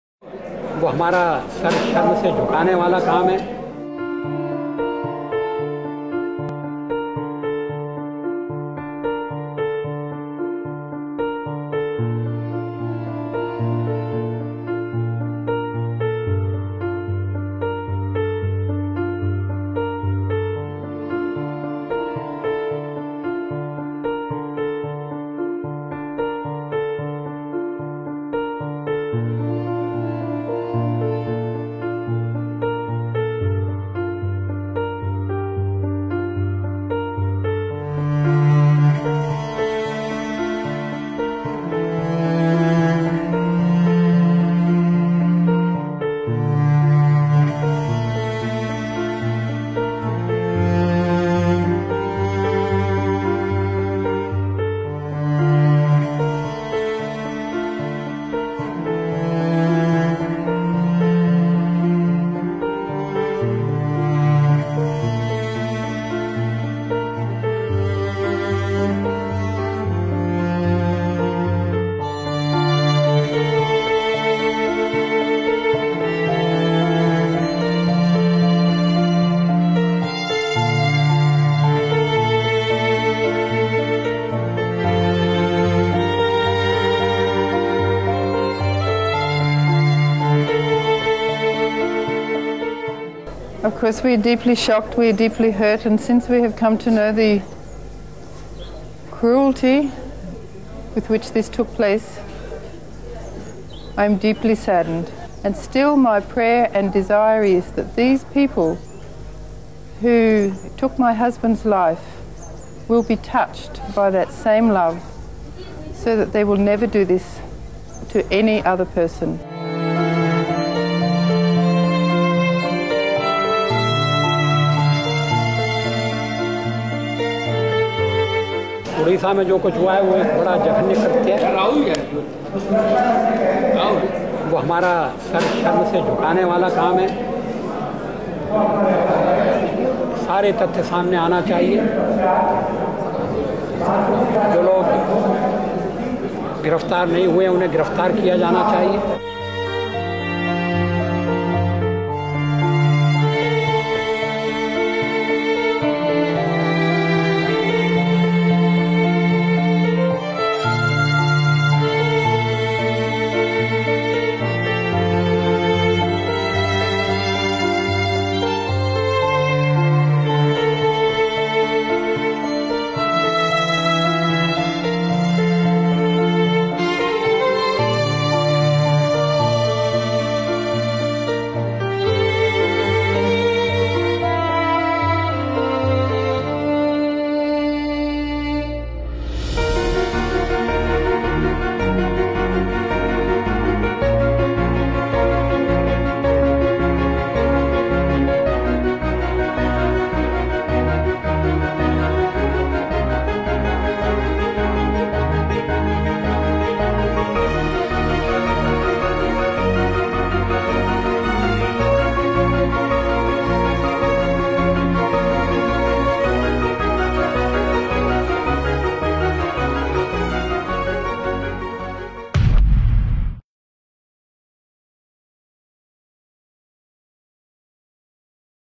Story of a Martior : Graham Stuart Stanes
A simple description about the great Missionary Graham Stuart Stanes..